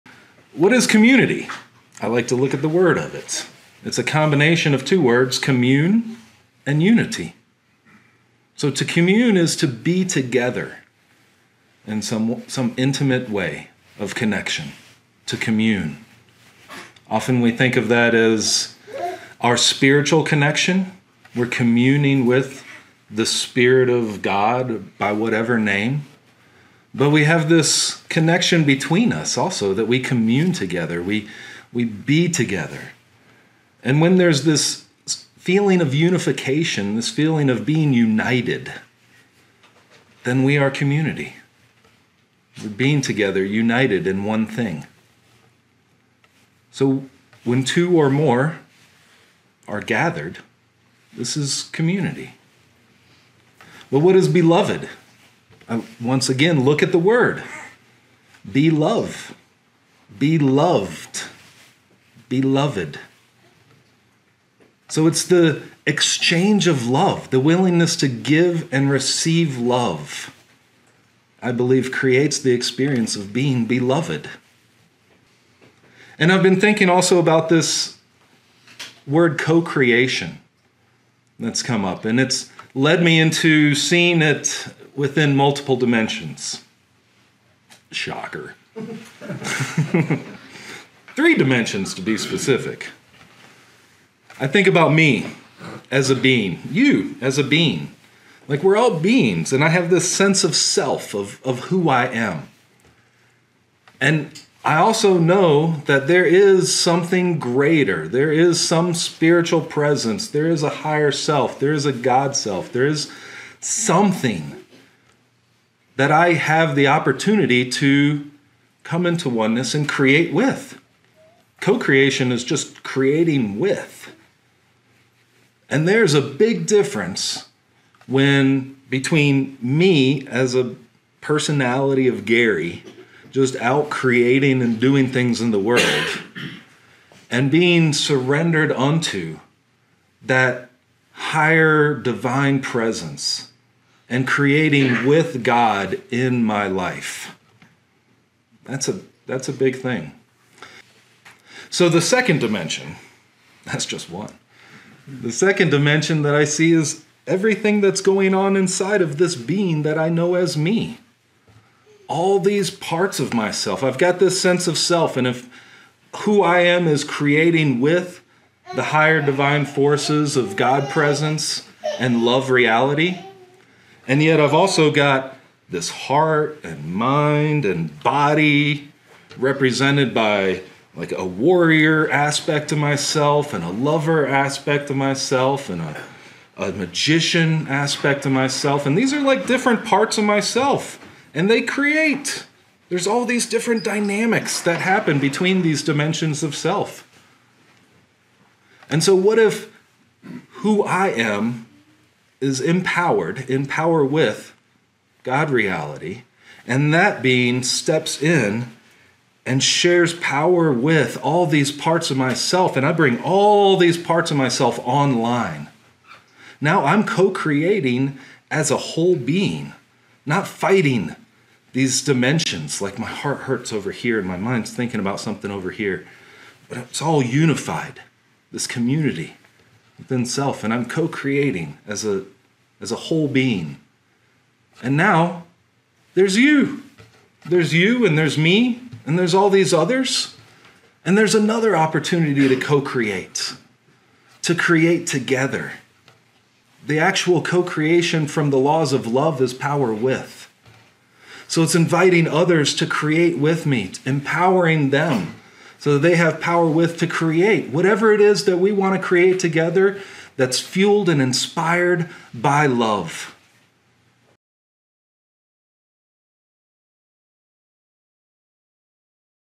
Gary Goodhue: Beloved Community - What Does It Mean?
🌟 Dive into the transformative concept of Beloved Community with Gary Goodhue in this enlightening excerpt from our Sunday Attunement service at Sunrise Ranch on March 24th, 2024. In this captivating discussion, Gary explores the essence of community as a beautiful blend of "commune" and "unity," underscoring the profound spiritual connection shared between individuals. Discover how the exchange of love and the willingness to give and receive love are foundational to the concept of being beloved.

💖 Join Gary as he delves into the notion of co-creation across various dimensions, from fostering self-awareness to aligning with a higher divine presence. Explore the dynamic interplay between different aspects of your being and the transformative power of co-creating with love at its core. Be inspired to empower others to join you in this collaborative journey of creation, driven by a deep-seated love for humanity and the world around us.

🤝 Don't miss out on this insightful exploration of Beloved Community—watch the full excerpt now and discover the transformative potential of communal love and co-creation. BelovedCommunity Community CoCreation SpiritualConnection Love SunriseRanch 🌿